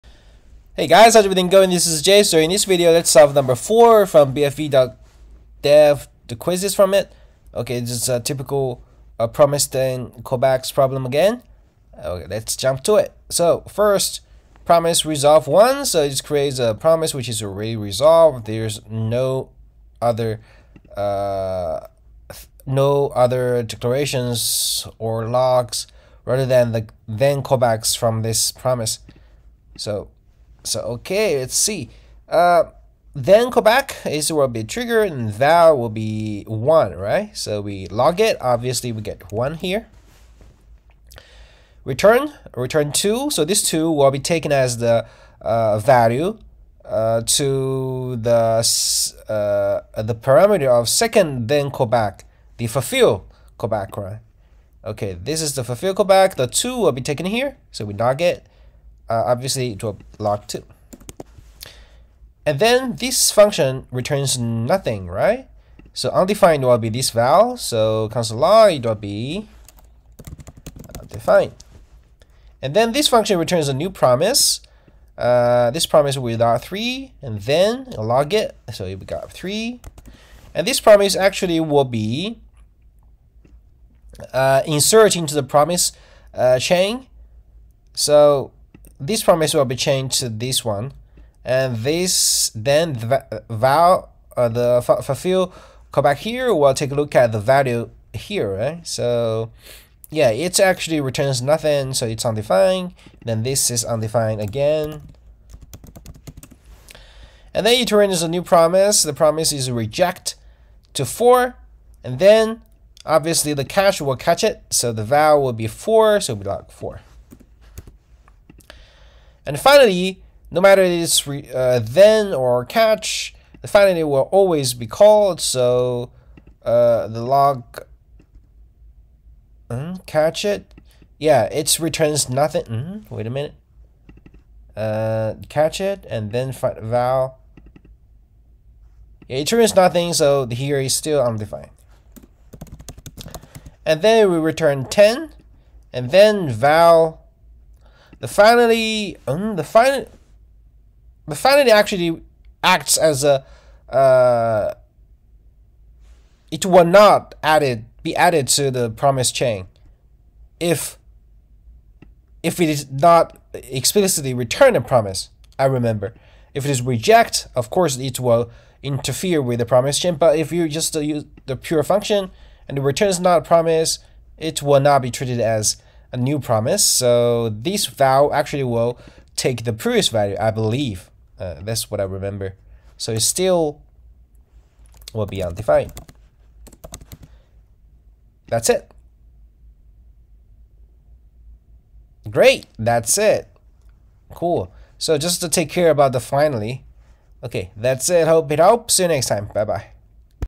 BFE.dev quiz #4 Promise Callback II | JSer - JavaScript lover
Not hard, just pay attention to finally()

and currently practicing leetCode & BFE.dev. If you are interested, maybe we can learn together.